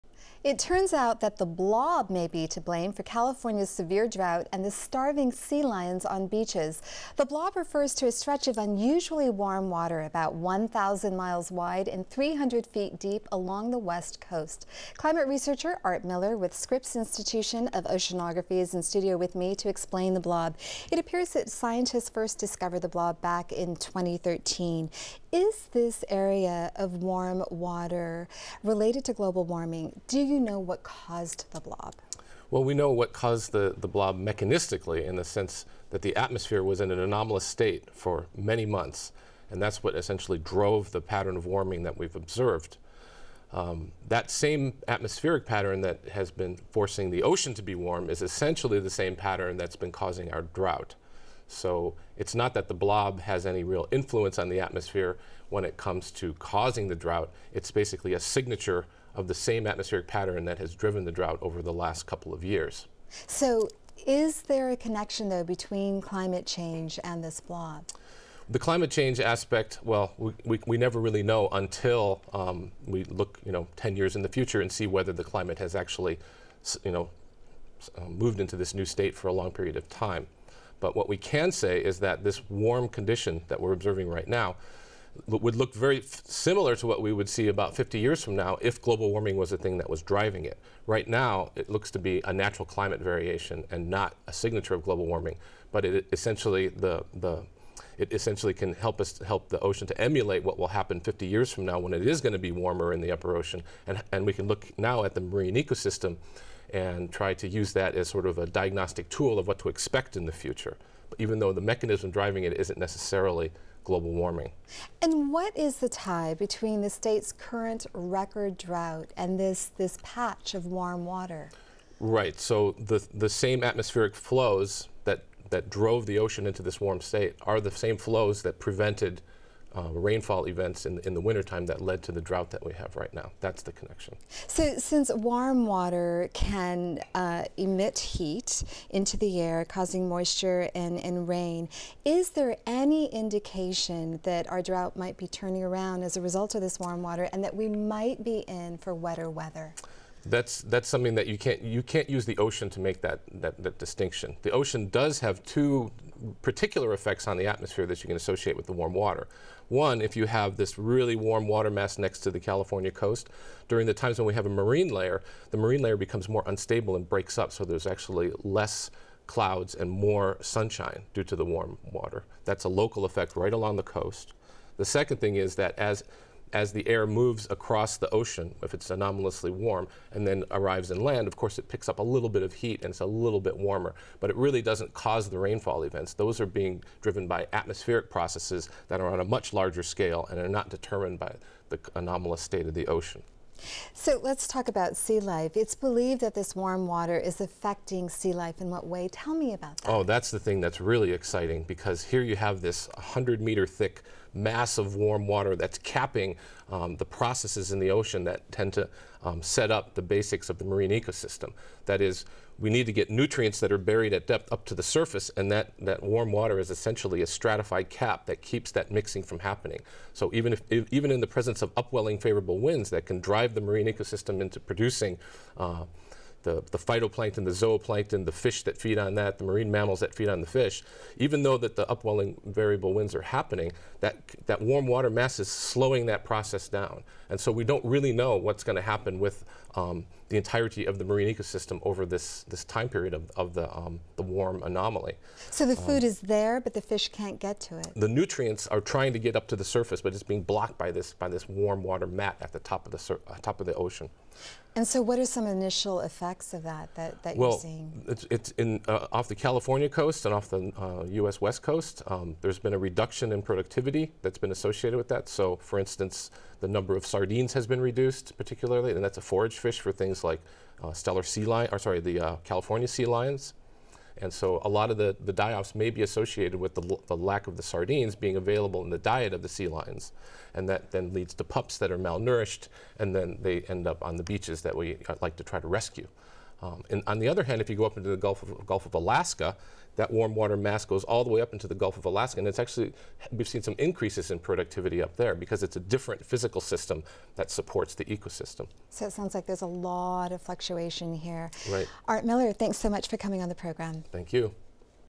Scientists Investigate Mysterious 'Blob' At UC San Diego
Questions about a mysterious mass of warm water in the Pacific Ocean and its effects on marine life bring researchers to the La Jolla campus.